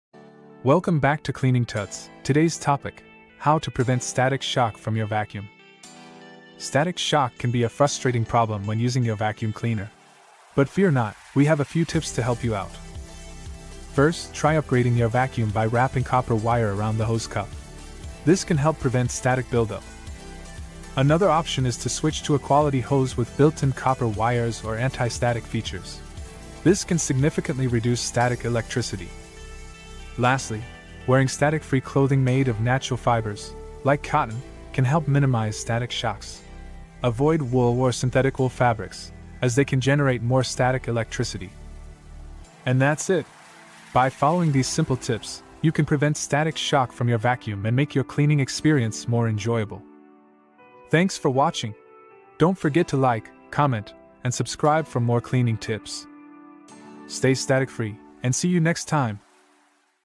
How to Prevent Static Shock from the Vacuum
Are you tired of getting static shocks from your vacuum? In this episode of CleaningTuts, we share simple and effective tips to prevent static buildup. Upgrade your vacuum, add moisture, and wear static-free clothing. Say goodbye to those annoying shocks!